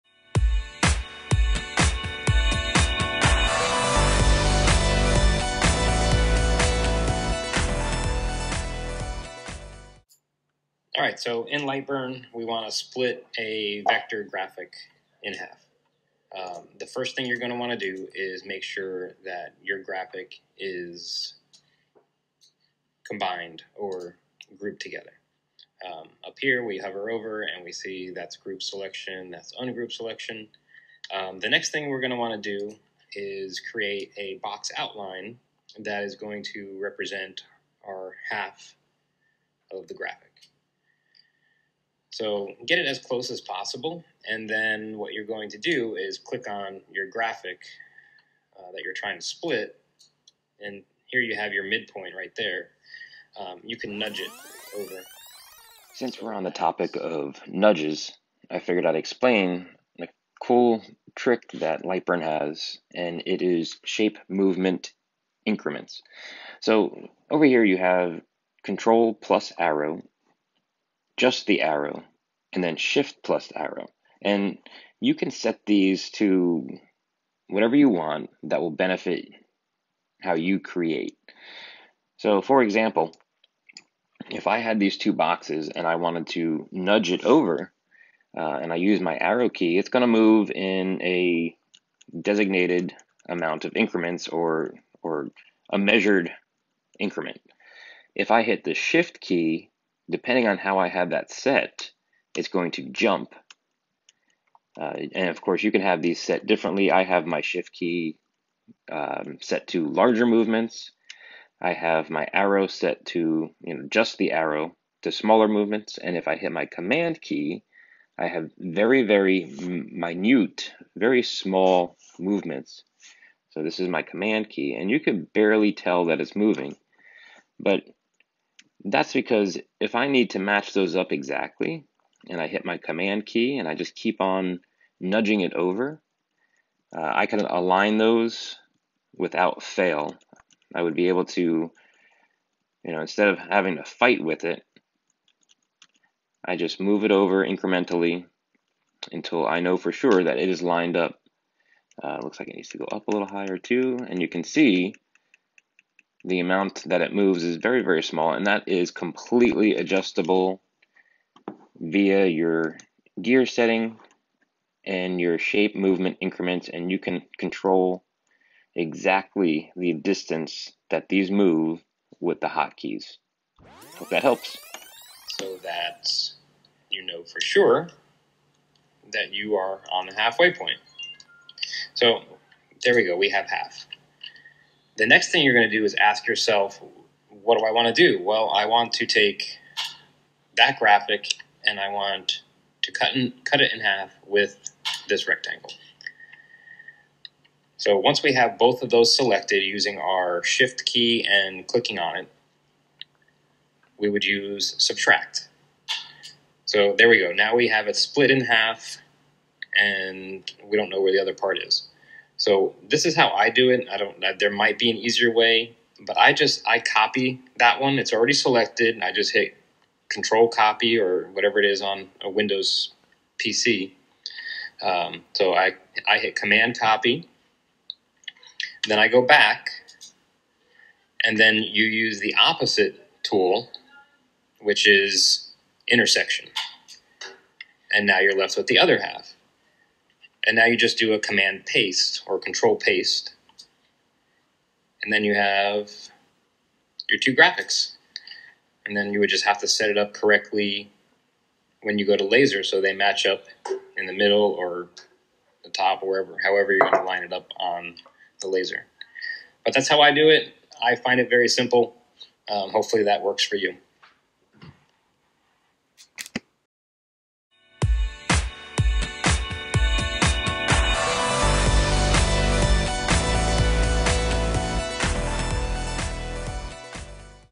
HOW TO.. Splitting graphics with Lightburn and some Nudging
Lightburn Splitting graphics and Nudging Else. How we try to make splitting graphics easy and how to use the "shape movement increments"  Thanks for tuning in and watching!